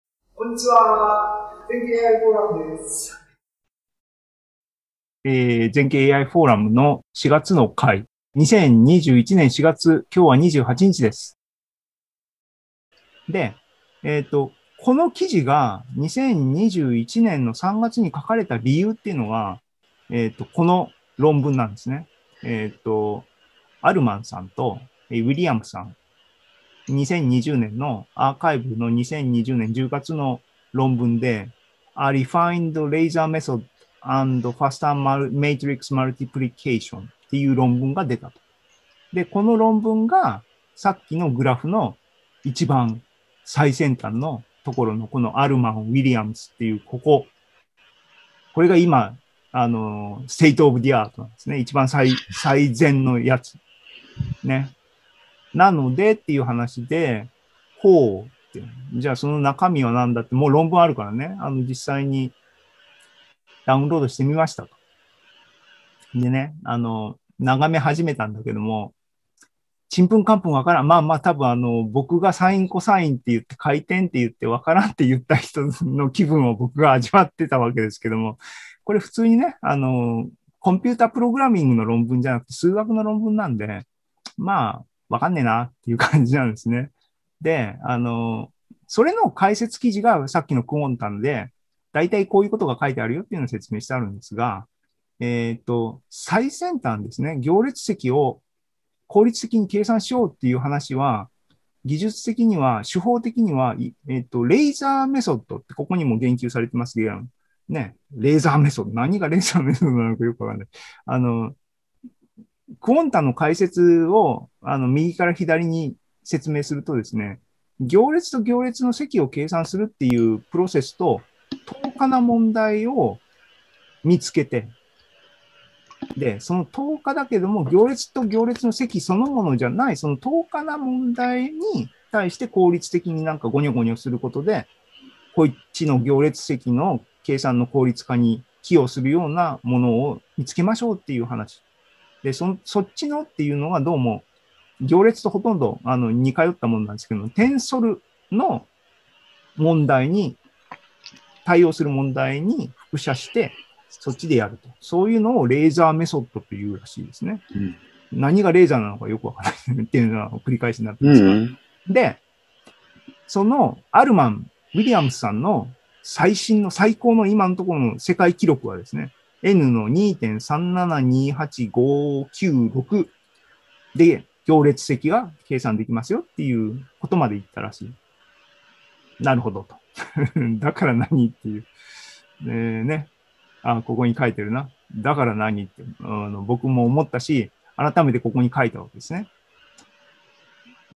『数理クイズ』解答編（４）レーザー・メソッド (ZAF-2104-10)
ZENKEI AI ポッドキャスト、シーズン１６は２０２１年４月２８日に開催した ZOOMライブの模様です。

この回はいちきの一人しゃべり、テーマは「ゴールデンウィークも AI」です。（ZOOM に Matt さん、ホンダさん、chan-mori さん、大島さん、米田さんも来てくれました。ありがとうございます。）

エピソード１０はメインの話題その１、前回の『数理クイズ』解答編の（４）Quanta で紹介されていた Alman, Vassilevska Williams による Laser Method についてです。

ZENKEI AI ポッドキャスト S16E10 に対応しています。

イベント全体の模様はアーカイブからご覧になれます。